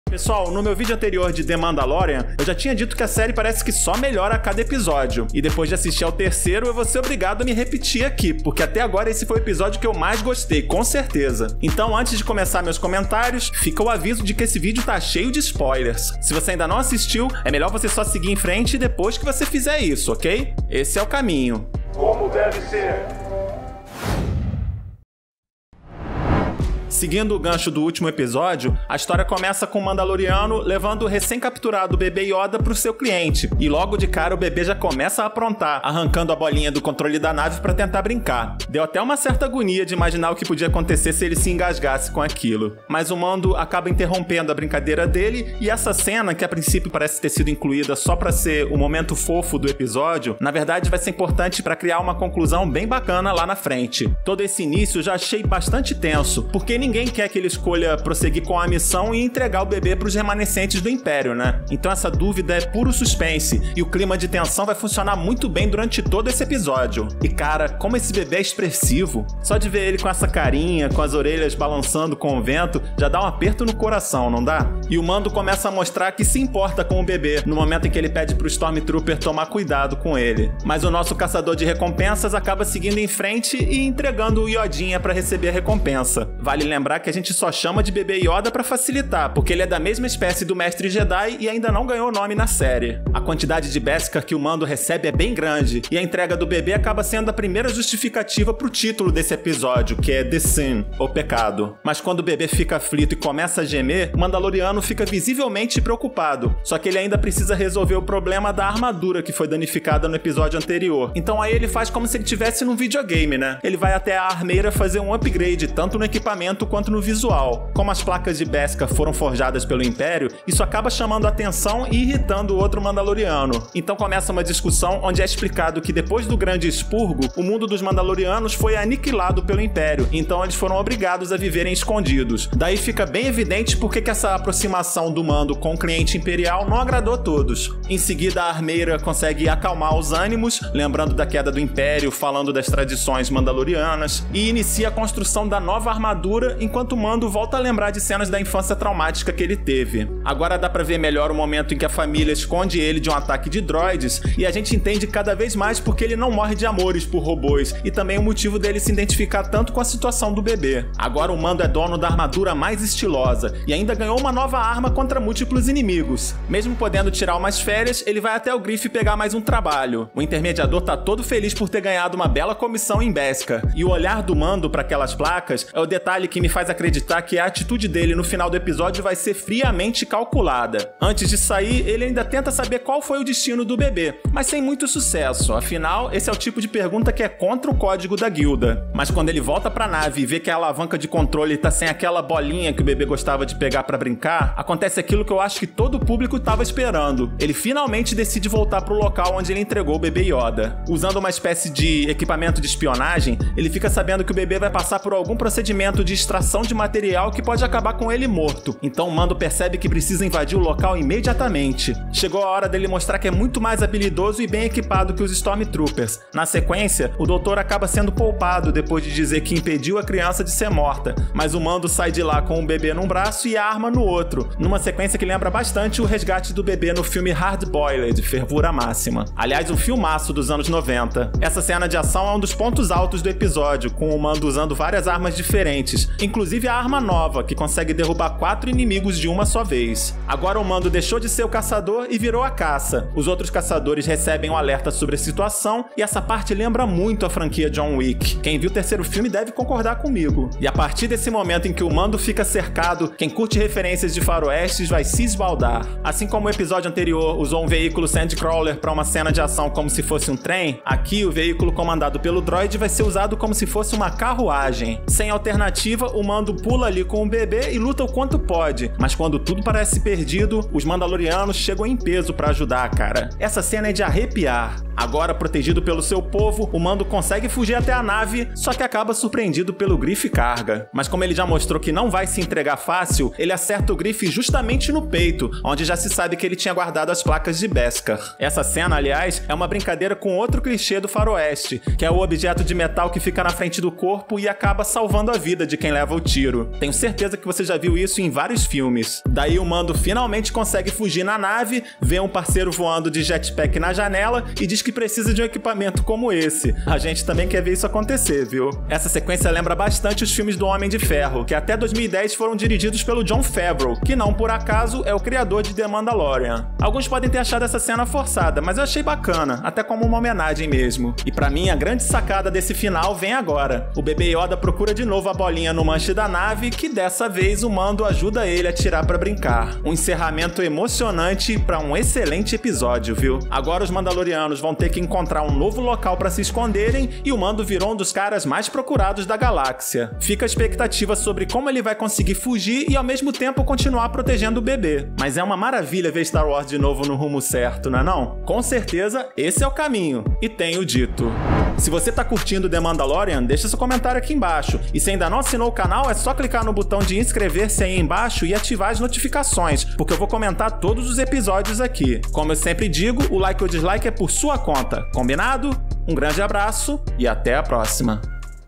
O Mandaloriano fica cada vez mais EMPOLGANTE! (Análise do EPISÓDIO 3 e algumas referências)
No meu vídeo anterior sobre The Mandalorian, eu já tinha dito que a série parece que só melhora a cada episódio. E depois de assistir ao terceiro, eu vou ser obrigado a me repetir aqui, porque até agora esse foi o episódio que eu mais gostei. "O Mandaloriano", até agora, está bom demais! :)